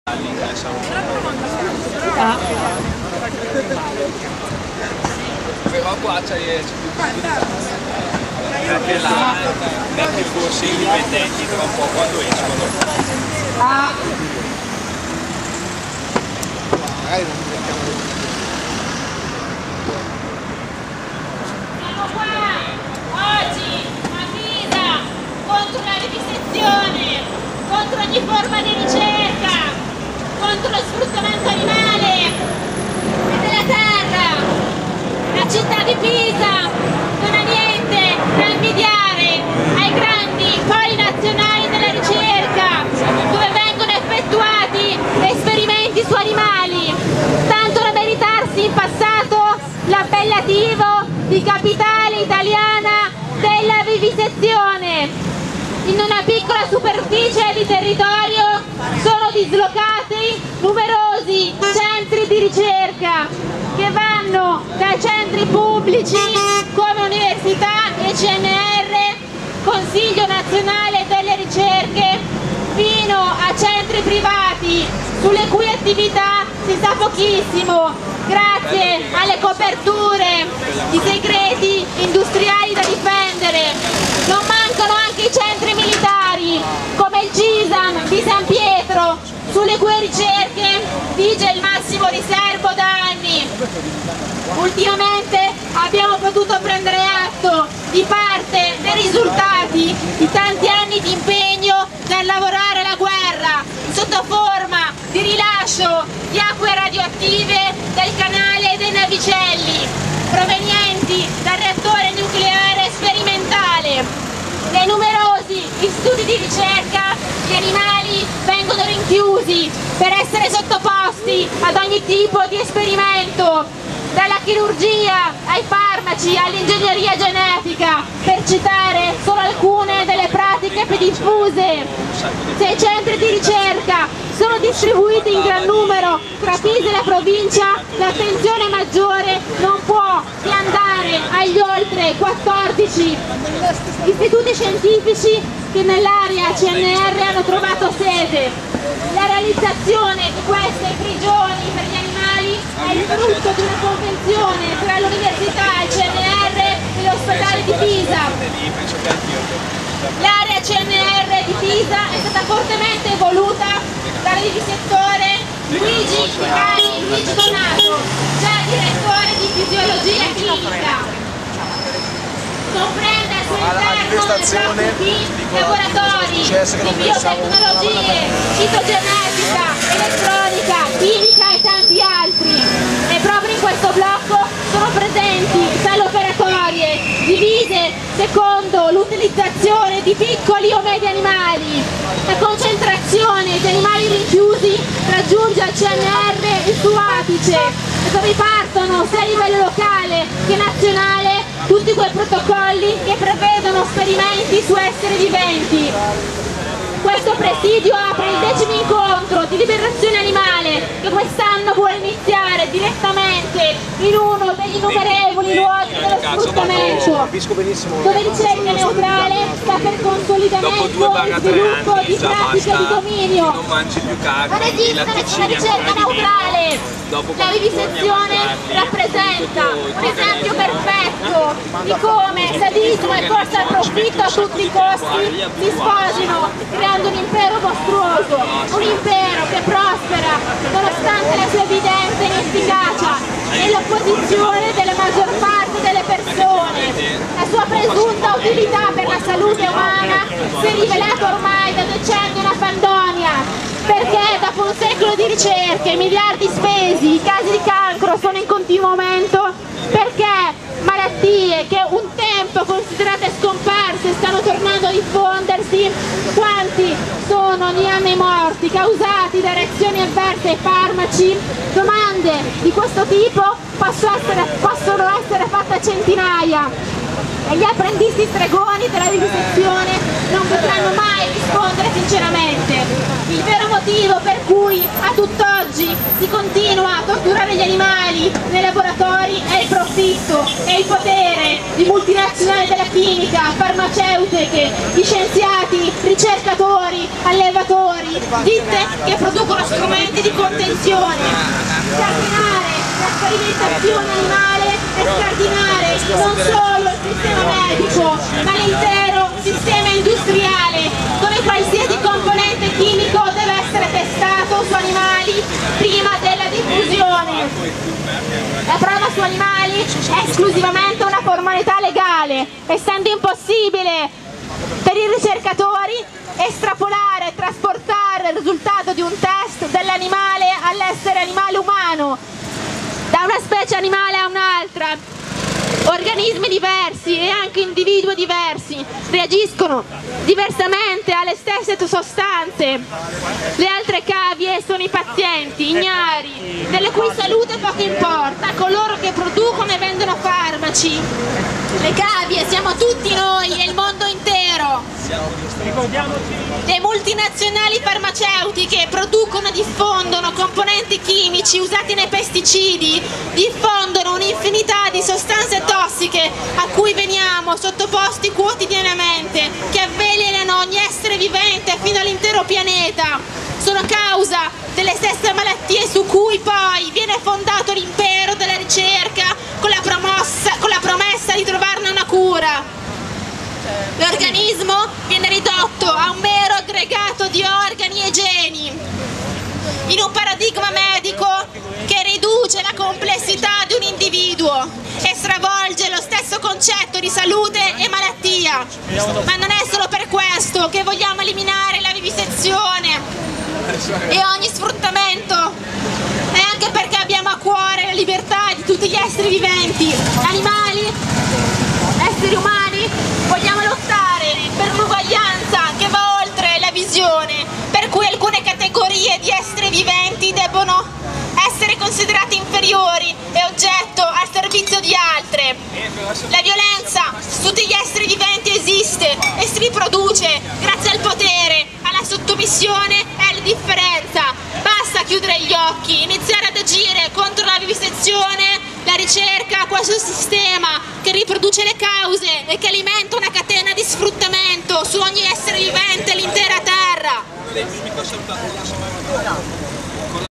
Presidio per le vittime della ricerca bio medica, CNR di Pisa 12 sett 2014
Il decimo incontro di Liberazione Animale al campeggio La Pinetina di Donoratico (LI) è iniziato "con un presidio diretto ad uno degli innumerevoli luoghi dello sfruttamento dove ricerca neutrale sta per consolidamento e sviluppo di pratiche di dominio" si legge nel programma della tre giorni. "La città di Pisa è tristemente nota per essere una delle capitali italiane della vivisezione con istituti privati e pubblici " (...) che trovano dimora presso il CNR ( ... ) "In questi luoghi si fanno esperimenti di ogni tipo: dalla chirurgia, alla testazione dei farmaci",
"Da questi luoghi partono sia a livello locale ( Università ) che nazionale protocolli che prevedono esperimenti dolorosi e invasivi su esseri viventi".

Qui un articoletto a cura di Canale 50 Pisa che in qualche maniera ha segnalato l'azione.